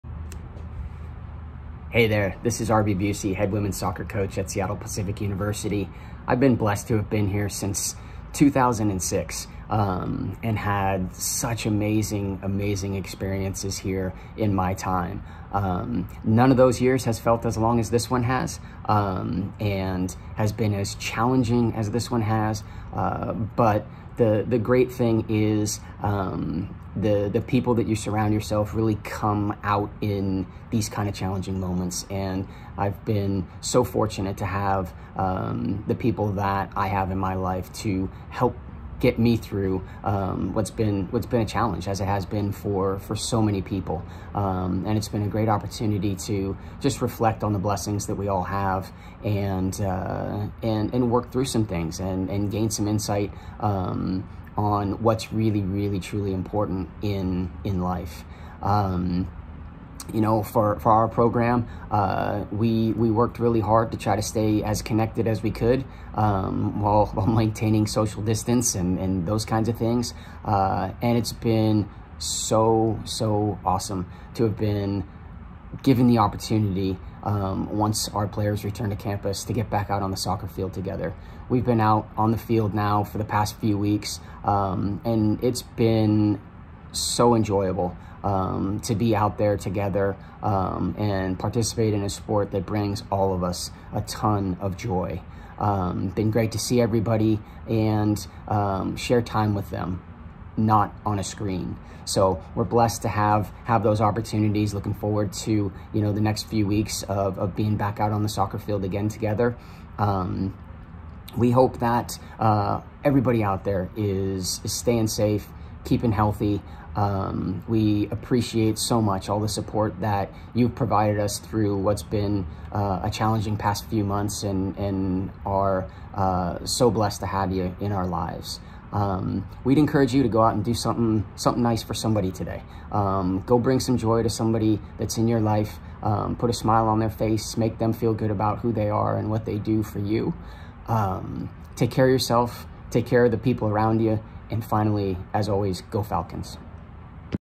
SPU SPORTS: Women's Soccer Coach Arby Busey (Oct. 29, 2020)
Seattle Pacific women's soccer coach Arby Busey talks about his tenure with the program and the challenging nature of the 2020 calendar year. He describes his teams efforts to connect in person after being apart for so many months.